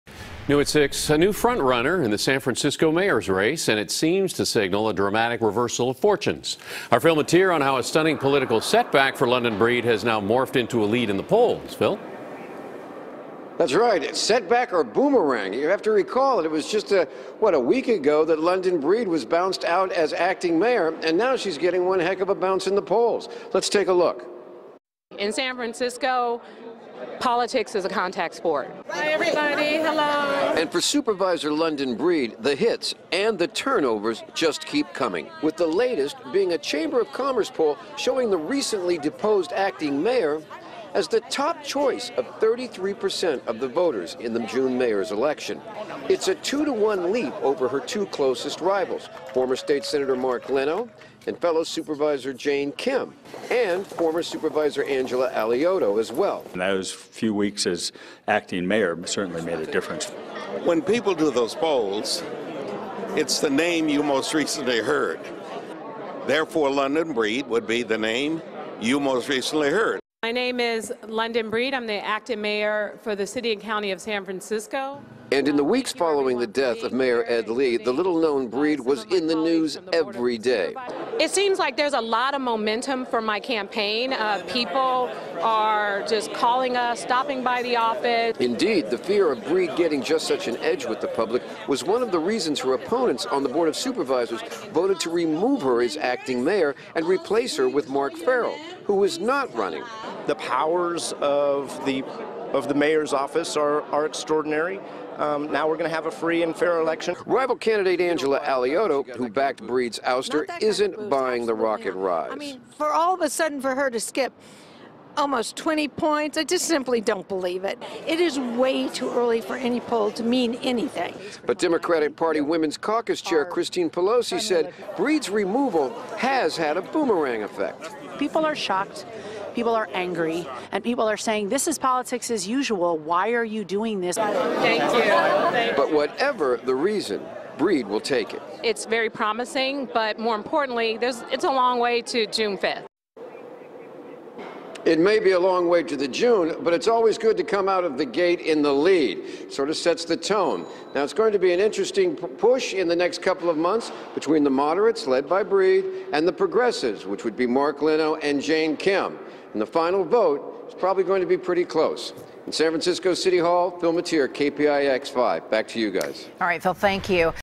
Poll Shows London Breed Is Frontrunner In SF Mayors Race
A new poll shows London Breed as the frontrunner in the SF mayoral race. Phil Matier reports on the sudden reversal of fortune.